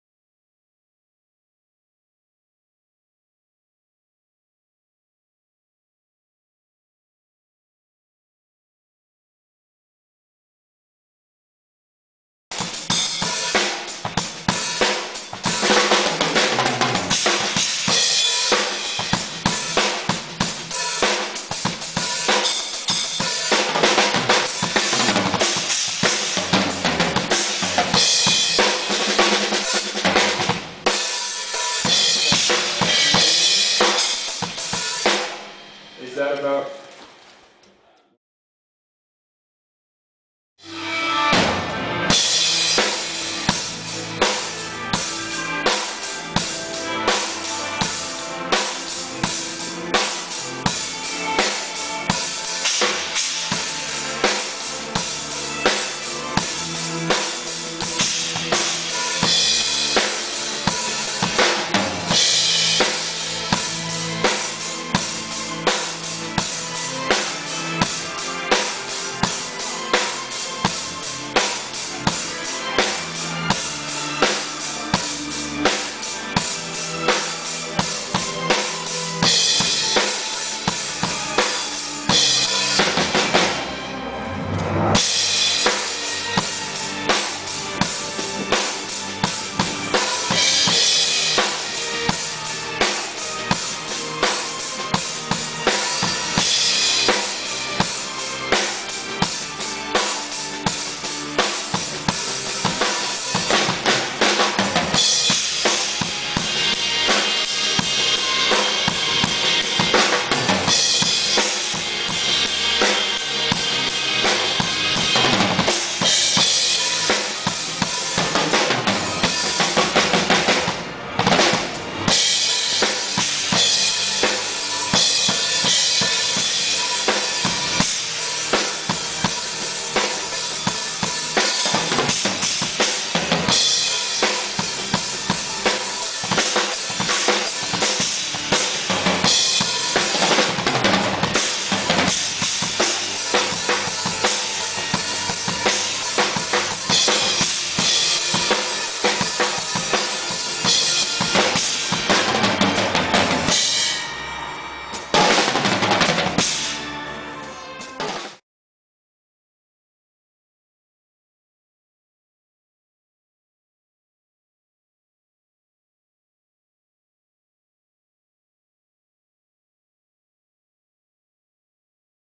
Nick Hatch - Sound check - Sweden 1
Drummer, Nick Hatch sound checking at a Ten Tenors show in sweden 2010.